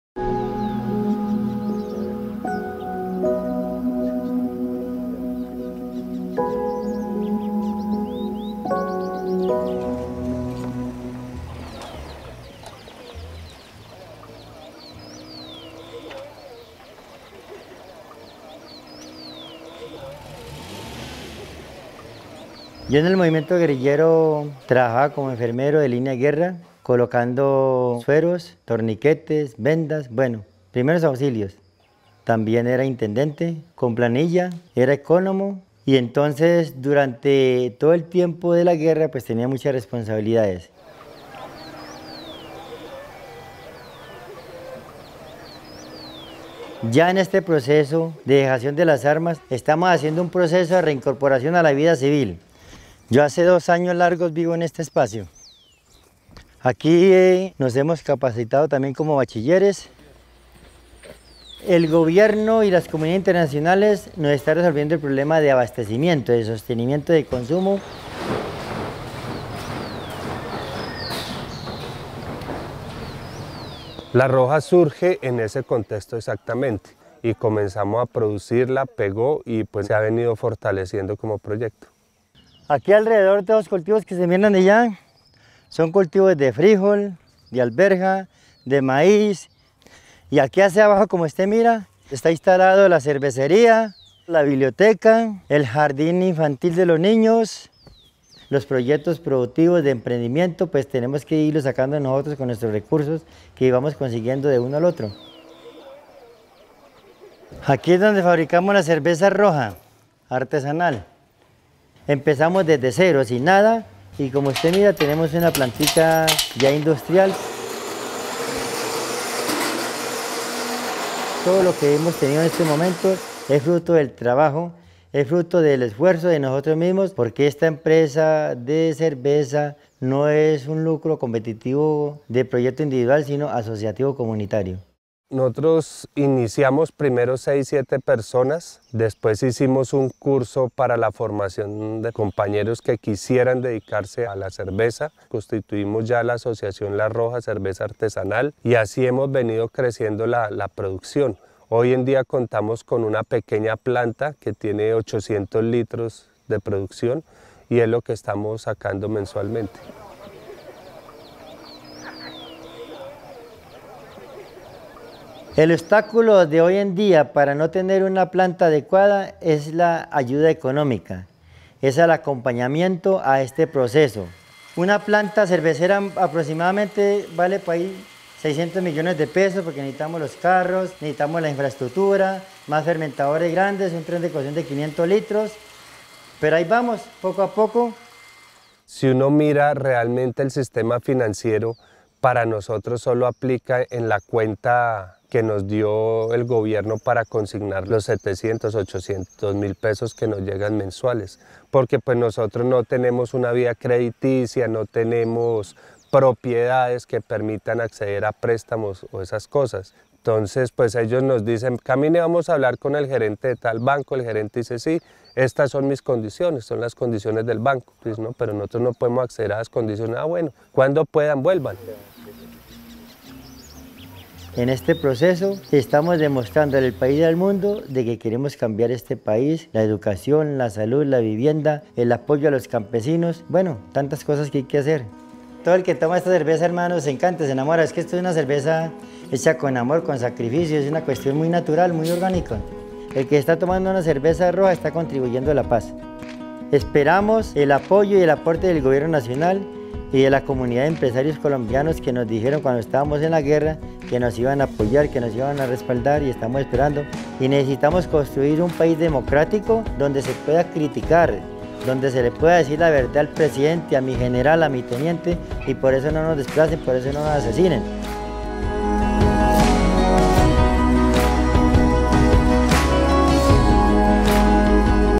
Identities of the World Colombia- Episode Three
Identities of the World- Ruben's Story

Ruben is a former FARC rebel living in Colombia. He has no credit or banking history which makes him financially invisible. Watch Ruben’s story to learn how financial exclusion impacts aspiring entrepreneurs.